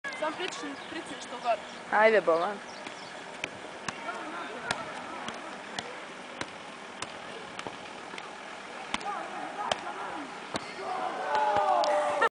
Sabrina Buljubasic s loptom
Me juggling but sadly my team-mate filmed it upside down... just joking around with my girls... salt city